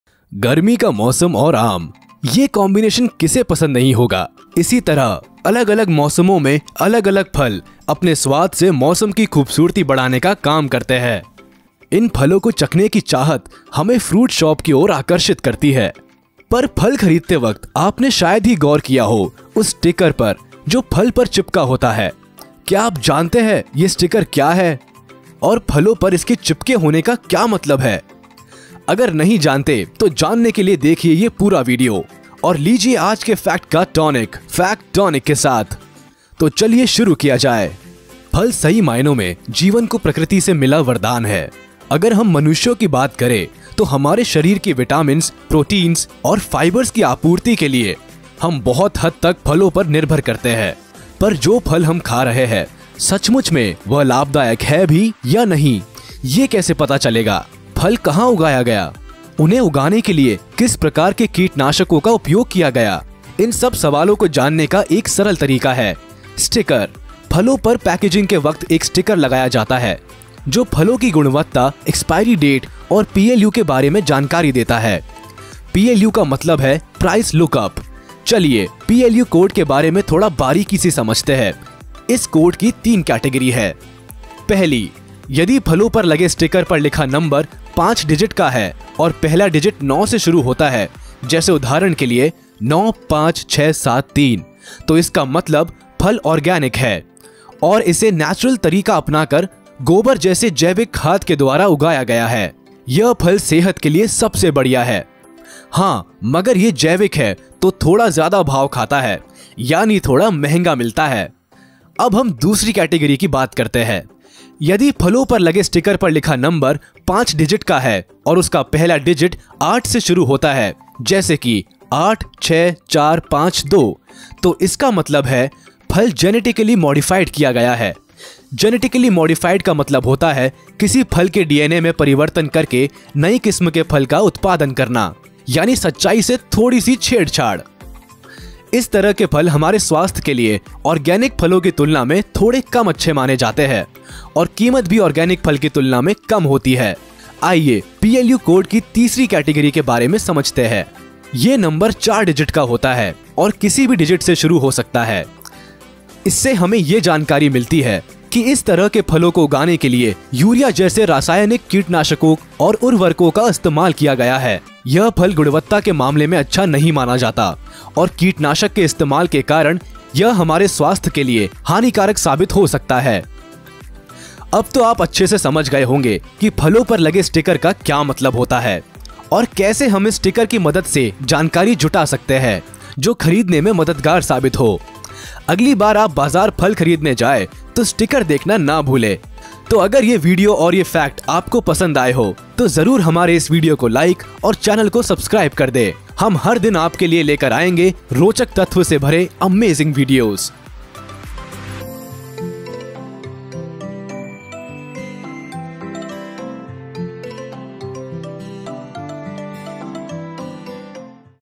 फल के ऊपर यह स्टीकर क्यों रहता है | Why Sticker on Fruit
आपने भी शायद फल खरीदते वक़्त फल के ऊपर चिपकाये गये एक स्टीकर देखा होगा और सोचा होगा की फल के ऊपर स्टीकर क्यों? आप ही नहीं, कही लोगो को इसके कारन मालुम नहीं होते। तो चलिए हम आपको बताते है फल के ऊपर स्टीकर होने का असली कारन।

You might also have noticed while buying fruit that they have a sticker on them! But why do fruits have stickers? What does the sticker mean? If you want the answers, watch the video as we have explained exactly why the stickers are glued to the fruits.